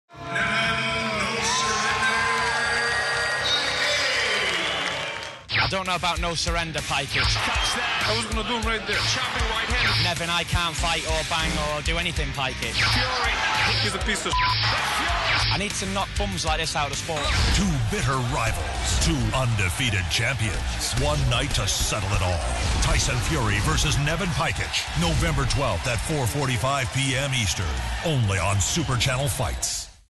TYSON FURY vs NEVEN PAJKIC - SAT NOV 12 - IN CANADA ONLY ON SUPER CHANNEL
Saturday November 12th, LIVE from EventCity in Manchester, England:
Undefeated heavyweight superstar Tyson Fury (16-0) puts his commonwealth title on the line against bitter rival and Canadian champion Neven "No Surrender" Pajkic, for the Commonwealth Heavyweight Championship.
Watch it LIVE in Canada starting at 4:45PM ET, ONLY on Super Channel.